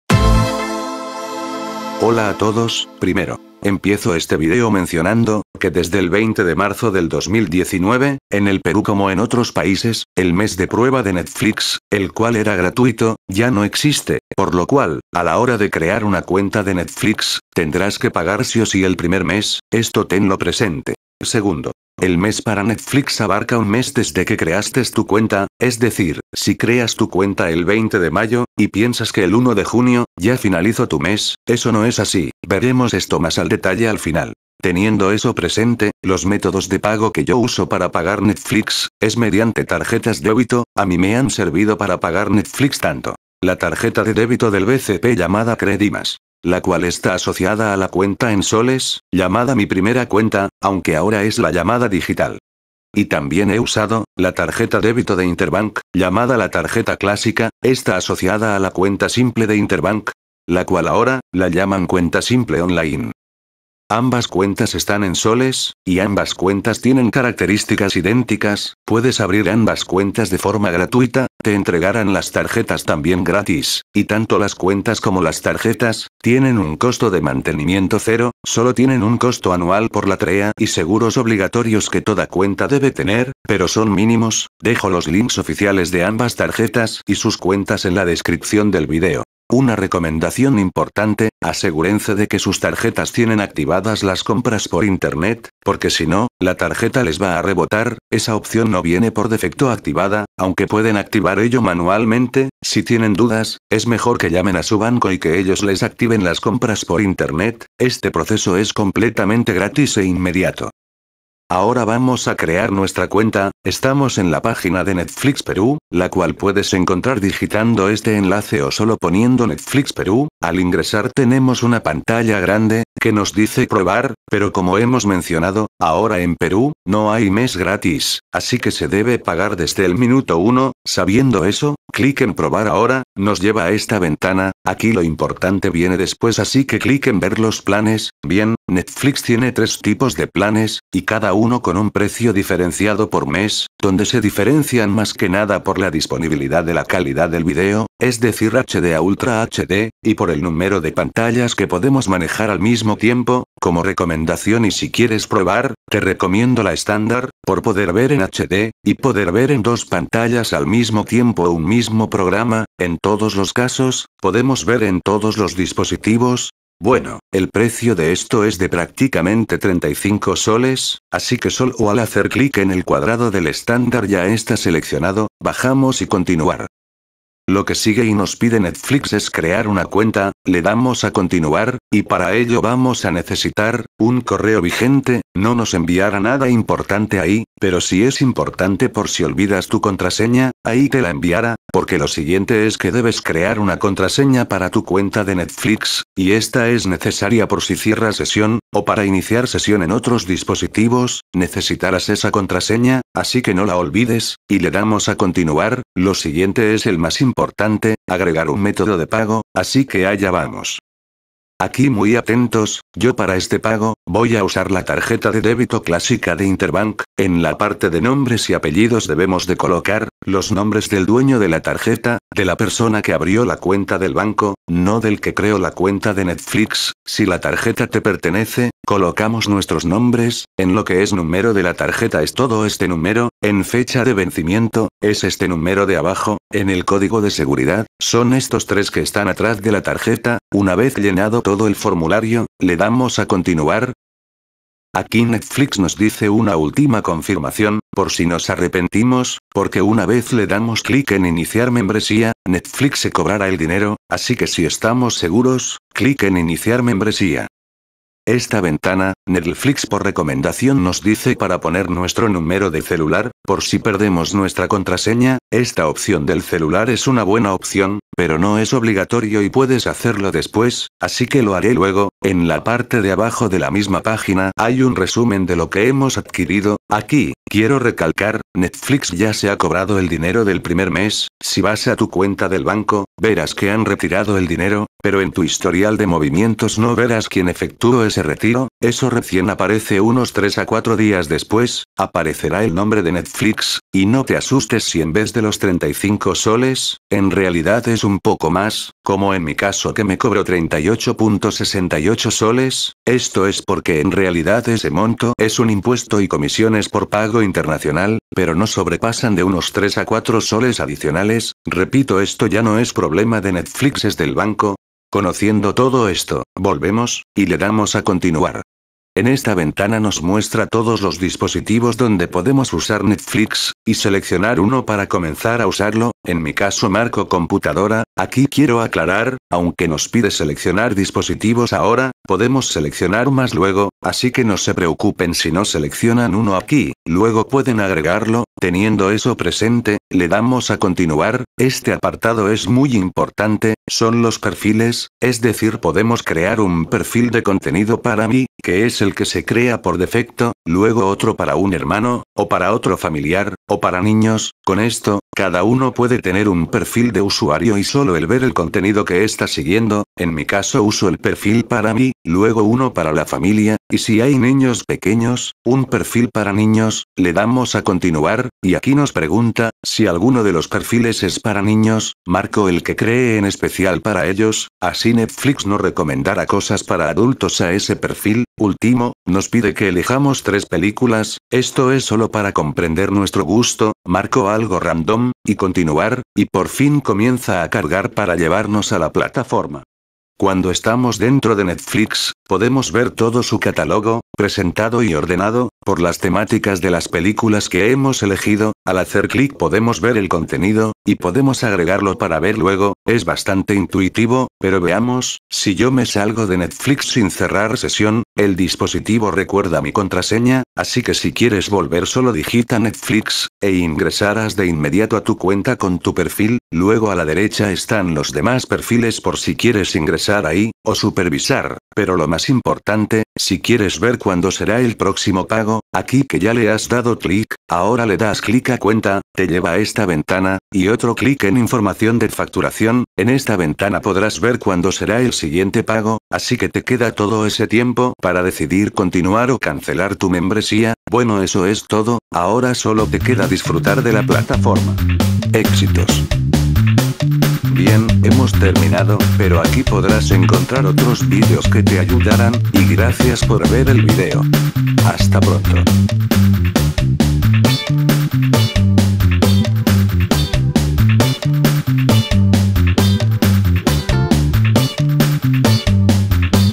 Tarjetas para pagar Netflix en Perú 2019 - Crear cuenta
Desde Marzo del 2019 en el Perú ya no existe el mes de prueba gratuito en Netflix, ahora desde el primer minuto se debe pagar el primer mes, veamos en el siguiente vídeo, que tarjetas de débito usar , recomendaciones y como crear una cuenta en netflix, todo ello a continuación.

▼SI, TE HA AYUDADO, APOYA AL CANAL CON UNA  DONACIÓN▼:
▼GRACIAS▼